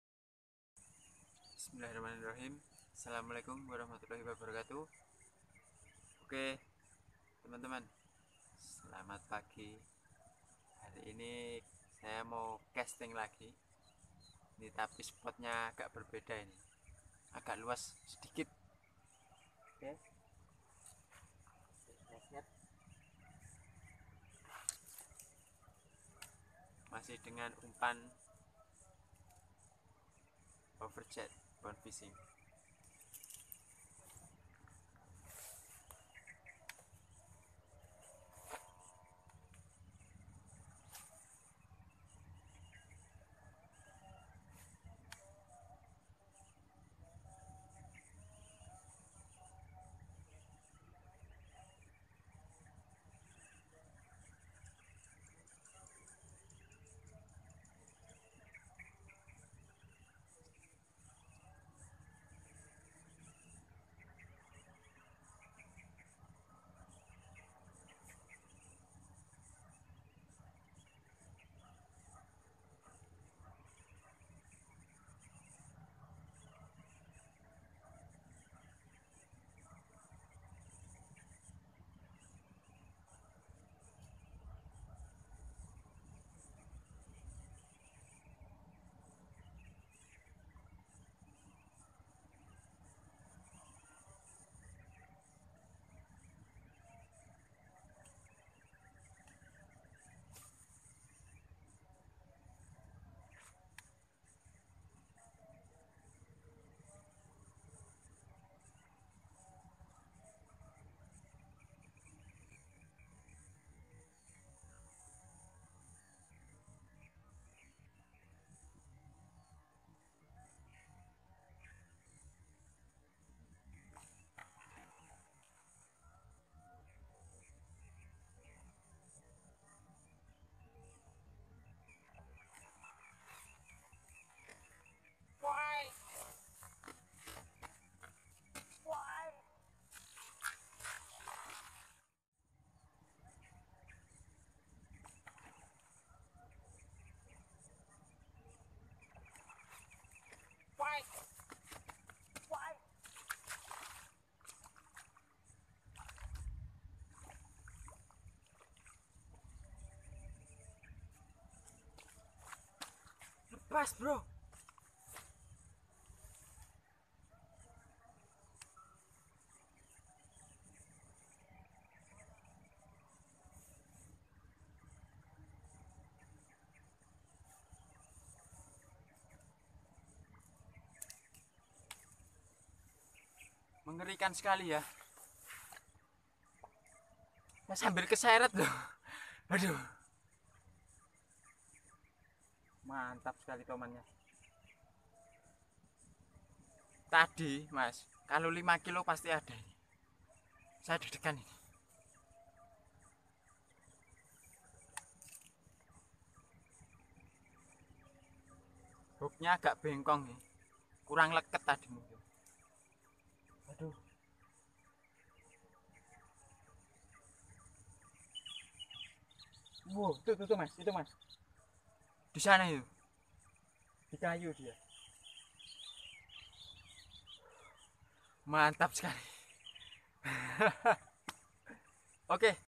BONE HOVERJET 130F | KEGANASAN TOMAN PAGI HARI SAMPAI DISERET | GIANT Snakehead Fish
Alhamdulillah bratherr..

Casting ikan gila
Snakehead Micropeltes